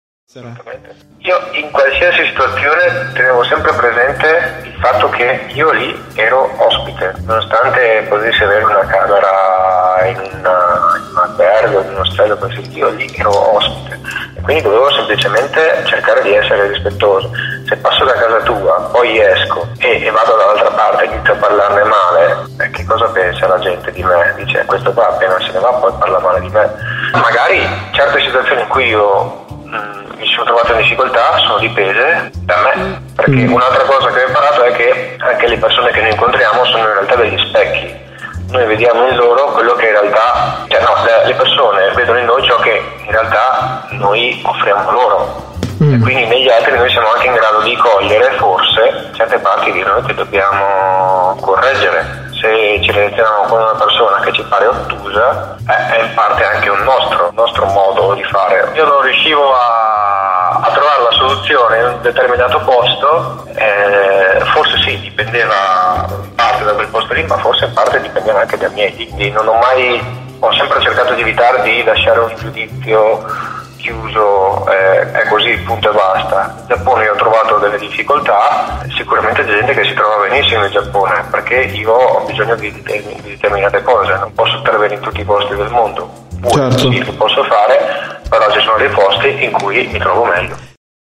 10.Intervista al viaggiatore cieco che ha fatto il giro del mondo: "Sentirsi ospite"
Un incidente di paracadutismo lo ha reso cieco a vent'anni, ma lui, Alessandro Bordini, non si è perso d'animo. Dopo aver preparato lo zaino, ha deciso di andarsene di casa, da solo, per fare il giro del mondo. "La gente è sostanzialmente buona" ha detto ai nostri microfoni "ero certo che mi avrebbe dato una mano per continuare a viaggiare". Ascoltate la sua bellissima avventura in questa intervista podcast.

La playlist è divisa in mini-parti estrapolate dalla conversazione.

Curatore della puntata: Simone Dabbicco
Intervistato: Alessandro Bordini